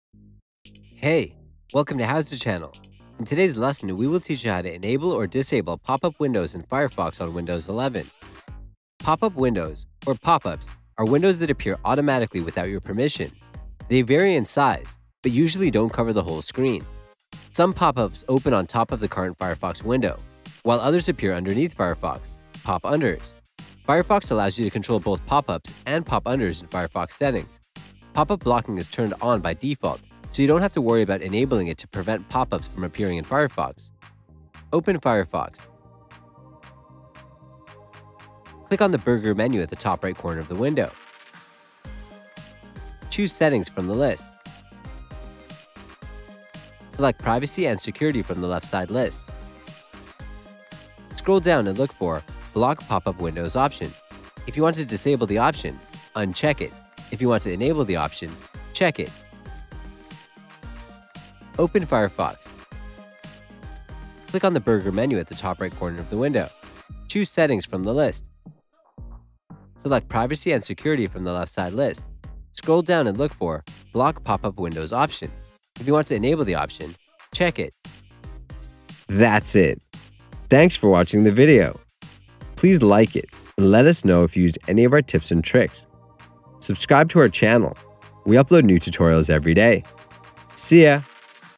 How to Enable or Disable Pop up Windows in Firefox on Windows 11
Hey! Welcome to HOWZA channel! In today's lesson, we will teach you how to enable or disable pop-up windows in Firefox on Windows11.
Pop-up windows, or pop-ups, are windows that appear automatically without your permission. They vary in size, but usually don't cover the whole screen. Some pop-ups open on top of the current Firefox window, while others appear underneath Firefox (pop-unders).
Firefox allows you to control both pop-ups and pop-unders in Firefox Settings . Pop-up blocking is turned on by default, so you don't have to worry about enabling it to prevent pop-ups from appearing in Firefox.
Open Firefox. Click on the burger menu at the top right corner of the window. Choose settings from the list.
Select Privacy and Security from the left side list. Scroll down and look for Block pop-up windows option. If you want to disable the option, uncheck it. If you want to enable the option – check it.
Select Privacy and Security from the left side list. Scroll down and look for Block pop-up windows option. If you want to enable the option – check it.